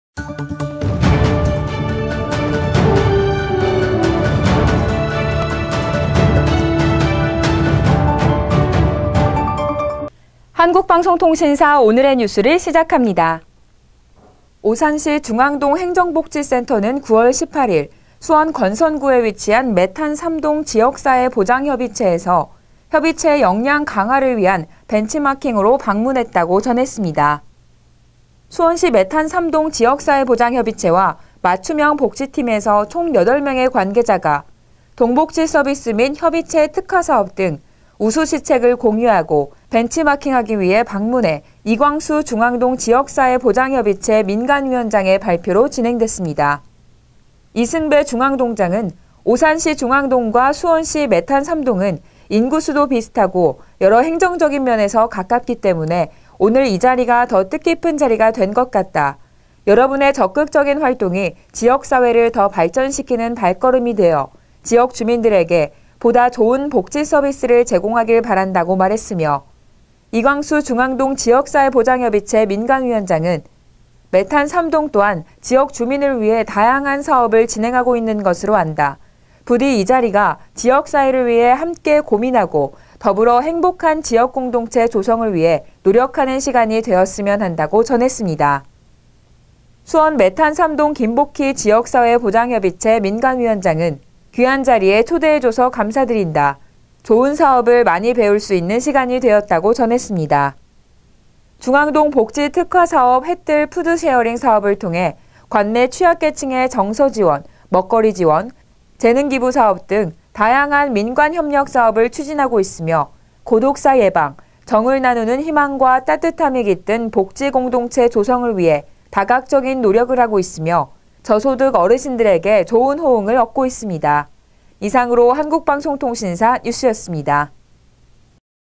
오산시, 수원 복지사업 우수 선진지 매탄3동 벤치마킹 방문
오산시 중앙동행정복지센터(동장 이승배)는 9월 18일 수원 권선구에 위치한 매탄3동 지역사회보장 협의체에서 협의체 역량강화를 위한 벤치마킹으로 방문하였다고 전했다.

수원시 매탄3동 지역사회보장 협의체(민간 위원장 김복희)와 맞춤형 복지팀에서 총 8명의 관계자가 동 복지서비스 및 협의체 특화사업 등 우수시책을 공유하고 벤치마킹하기 위해 방문하여 이광수 중앙동 지역사회보장 협의체 민간위원장의 발표로 진행되었다.

이승배 중앙동장은 “오산시 중앙동과 수원시 매탄3동은 인구수도 비슷하고 여러 행정적인 면에서 가깝기 때문에 오늘 이 자리가 더 뜻깊은 자리가 된 것 같다. 여러분의 적극적인 활동이 지역사회를 더 발전시키는 발걸음이 되어 지역 주민들에게 보다 좋은 복지서비스를 제공하길 바란다.”라고 하였고,
이광수 중앙동 지역사회보장 협의체 민간위원장은 “매탄3동 또한 지역주민을 위해 다양한 사업을 진행하고 있는 것으로 안다. 부디 이 자리가 지역사회를 위해 함께 고민하고 더불어 행복한 지역 공동체 조성을 위해 노력하는 시간이 되었으면 한다.”라고 전했다.

수원 매탄3동 김복희 지역사회보장 협의체 민간위원장은 “귀한 자리에 초대해줘서 감사드린다. 좋은 사업을 많이 배울 수 있는 시간이 되었다.”라고 전했다.

중앙동 복지 특화사업“해 뜰 푸드쉐어링 사업”을 통해 관내 취약계층의 정서지원, 먹거리 지원, 재능기부 사업 등 다양한 민관협력사업을 추진하고 있으며, 고독사 예방, 정을 나누는 희망과 따뜻함이 깃든 복지공동체 조성을 위해 다각적인 노력을 하고 있으며, 저소득 어르신들에게 좋은 호응을 얻고 있다.